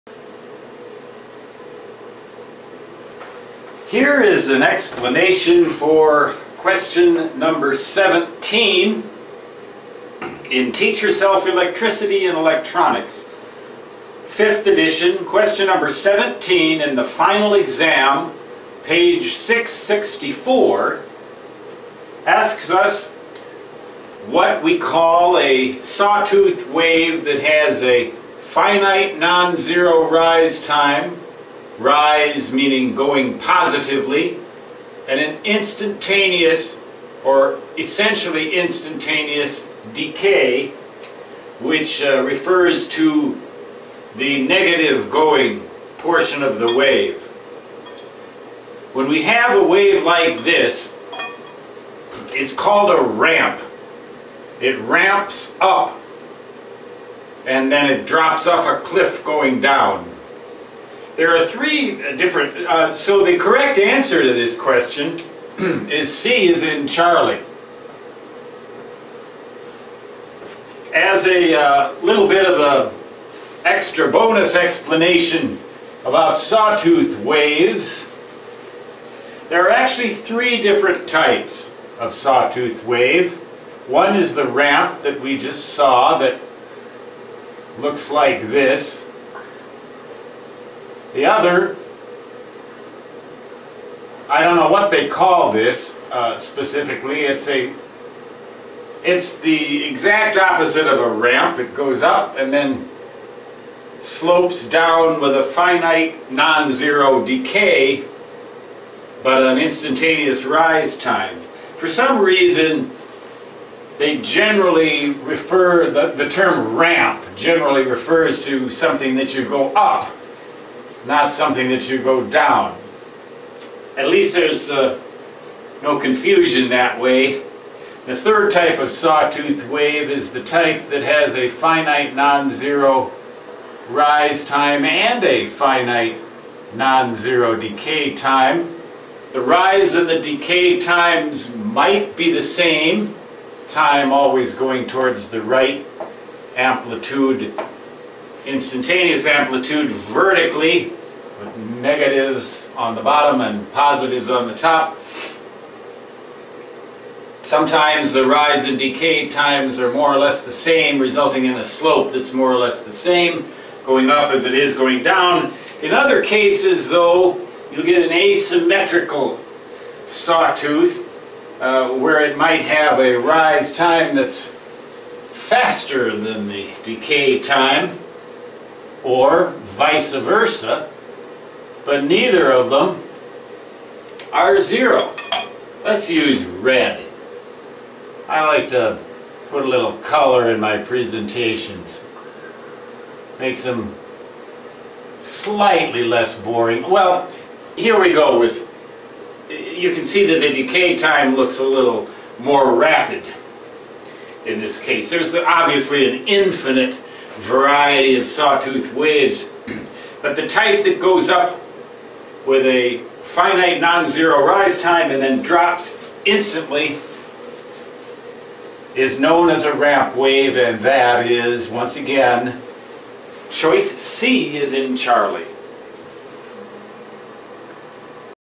Teach Yourself EE Exam Q 17
Explanation for the answer to Final Exam Question No. 17 in "Teach Yourself Electricity and Electronics, 5th Edition" (page 664).